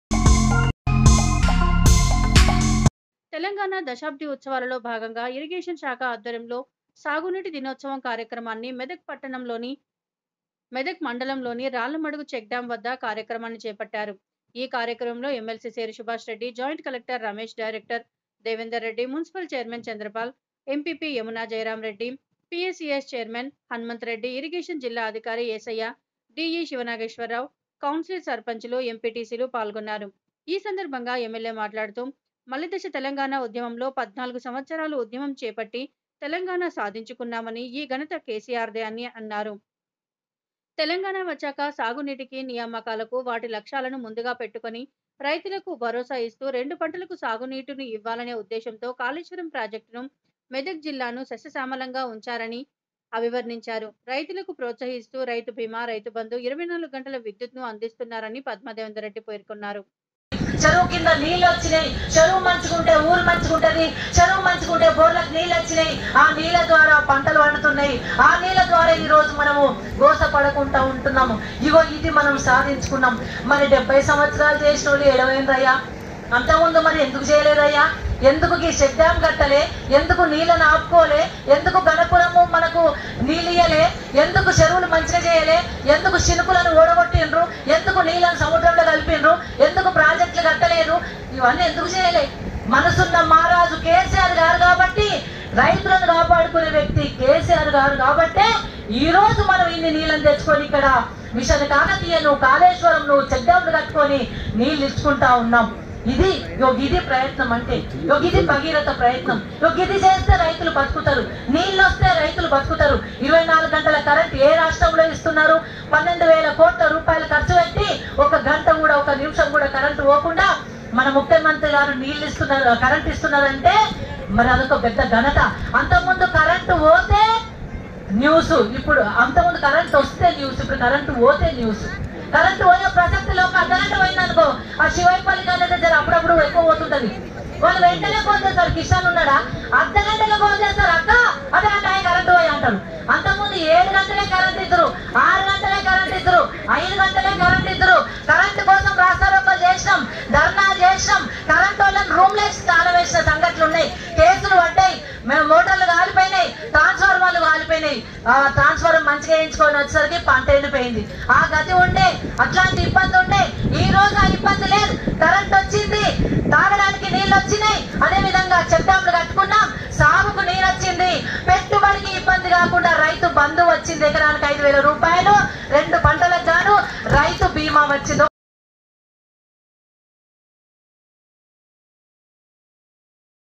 మెదక్ ర్యాలమడుగు గ్రామంలో సాగు నీటి దినోత్సవ కార్యక్రమం లో పాల్గొన్న ఎమ్మెల్యే పద్మ దేవేందర్ రెడ్డి
మెదక్ మండలం  ర్యాలమడుగు గ్రామంలో సాగు నీటి దినోత్సవ కార్యక్రమం లో పాల్గొన్న ఎమ్మెల్యే పద్మ దేవేందర్ రెడ్డి